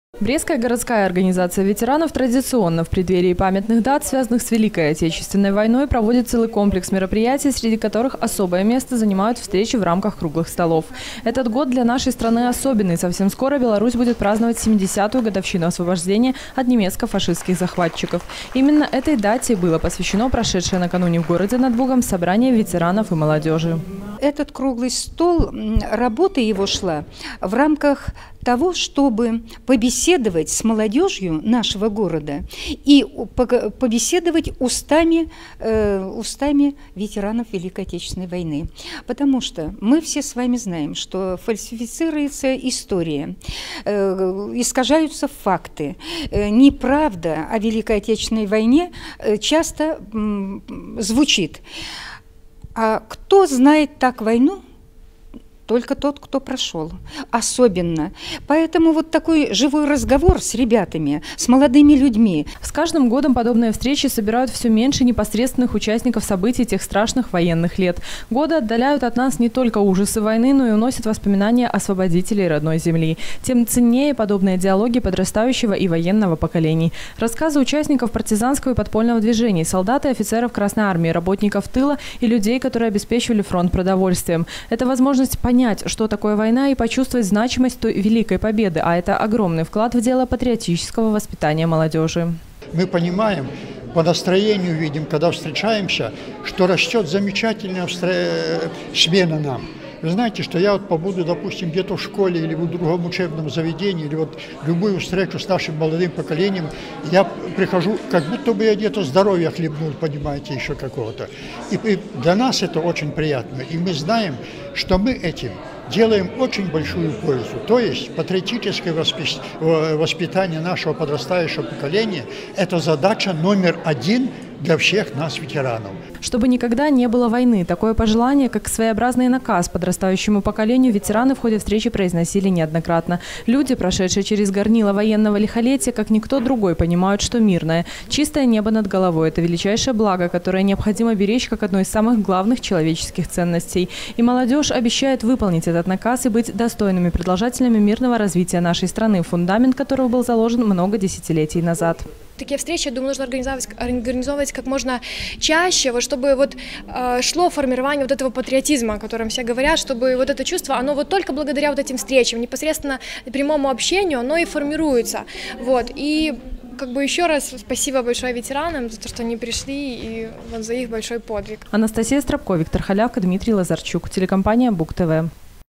2014-06-12 г. Брест Телекомпания Буг-ТВ. Встреча ветеранов Великой Отечественной войны и молодежи
Встреча  ветеранов Великой Отечественной войны и молодежи  за круглым столом в Брестском горисполкоме «70-летию освобождения Беларуси посвящается. Операция Багратион».
Любовь к Отчеству, преданность ему и стремление своими действиями и поступками служить его интересам -- это только несколько основных постулатов, которые объединены под словом патриотизм.
Быть настоящим патриотом не значит говорить об этом постоянно, быть патриотом -- значит  творить конкретные дела: важные, стоящие, благородные. Примеры такой беззаветной любви к родному краю нам, белорусам, не нужно долго искать -- ветераны Великой отечественной войны - самый главный образец и эталон верности Родине. Даже просто находясь рядом, они помогают понять, каким должен быть настоящий гражданин и патриот. А потому встречи с ветеранами так важны для подрастающего поколения. В городе над Бугом они уже не один год проводятся под эгидой Брестской городской ветеранской организации. На круглом столе, проведённом накануне, побывала и наша съёмочная группа.